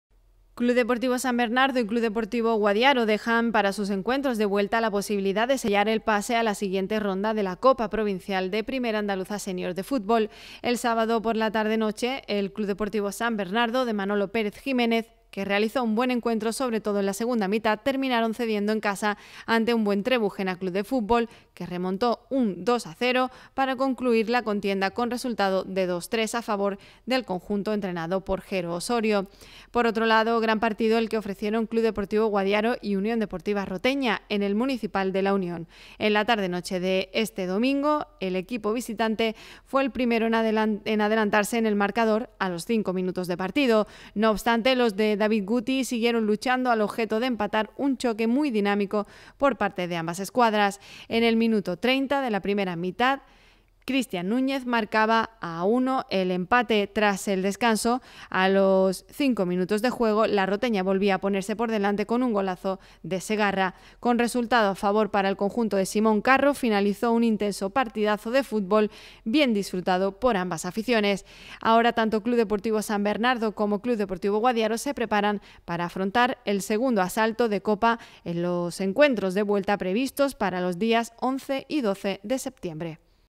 NOTICIA FUTBOL COPA PROVINCIAL 6 SEPTIEMBRE 2021
Arrancaron este fin de semana los encuentros de cuartos de final de la Copa Provincial de Primera Andaluza Sénior de Fútbol, donde están encuadrados los equipos sanroqueños del CD San Bernardo y CD Guadiaro. Ambos cayeron ante sus respectivos rivales, el Trebujena CF y la UD Roteña, en sendos partidos de mucha calidad.